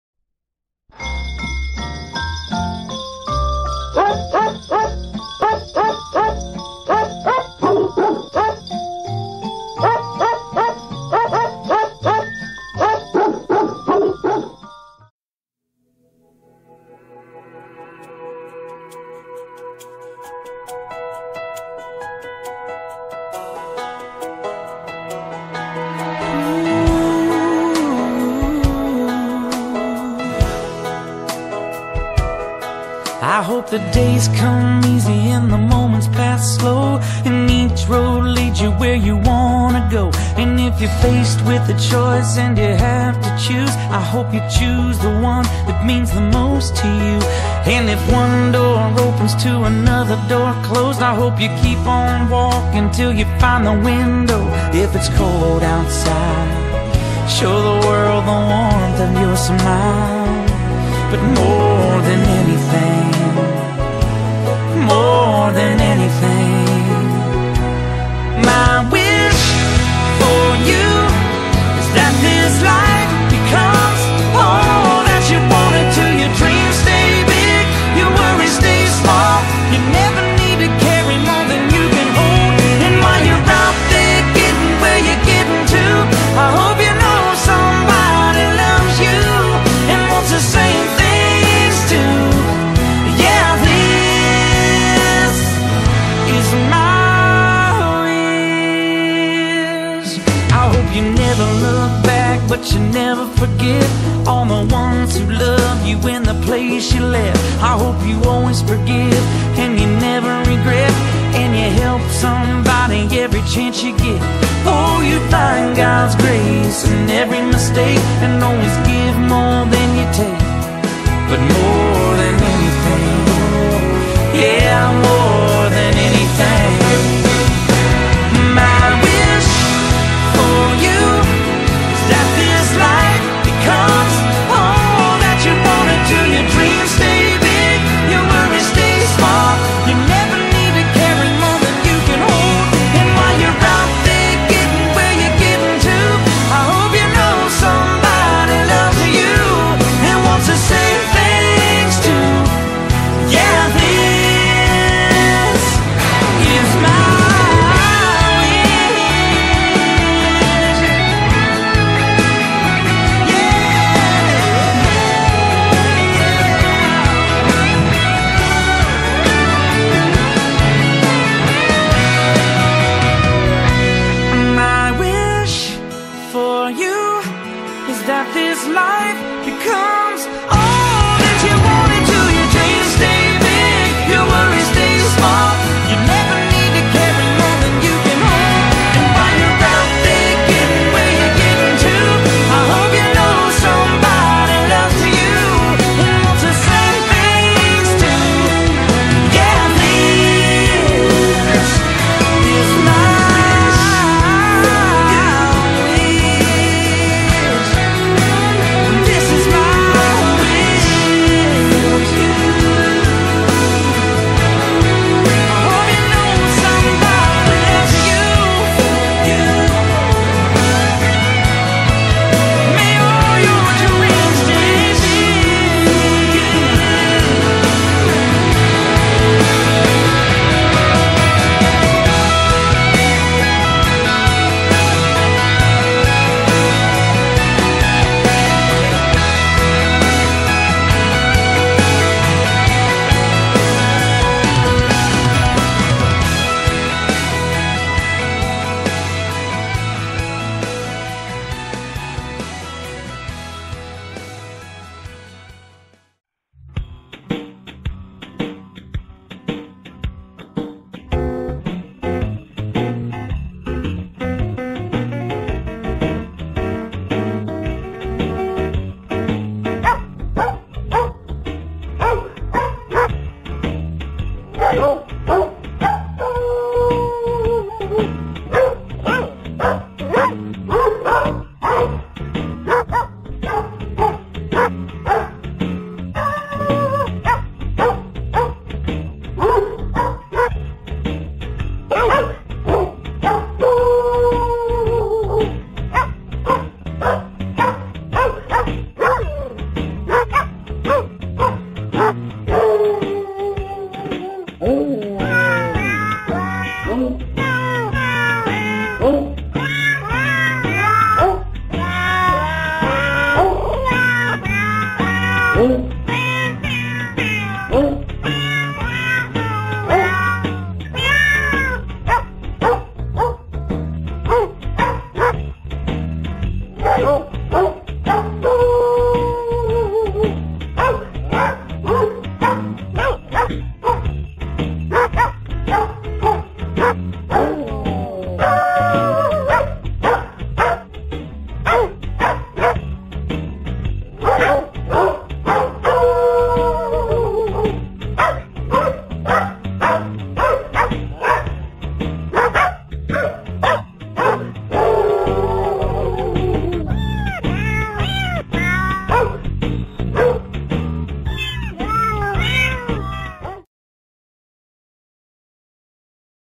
MY CHRISTMAS WISH FOR YOU ... My Family & Friends
My Wish for you this Christmas Season ...
May the Spirit of Christmas live within your heart all year!
Peace.  Love.  Light. Happiness.  Good Health.  Memories to last a life time.
And May You Always .BELIEVE!!